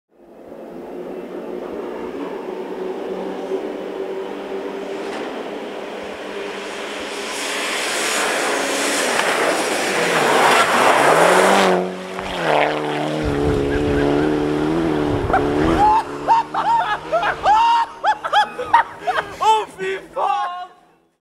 Henning Solberg living up to his sponsor "Go Fast" - Motorsportfilmer.net
Henning Solberg going flat out trying to catch Mikko Hirvonen on Rally Finnskog in Norway. The last rally of this car was Finland 2001 with Tommi Mäkinen, he also won Safari Rally with the same car.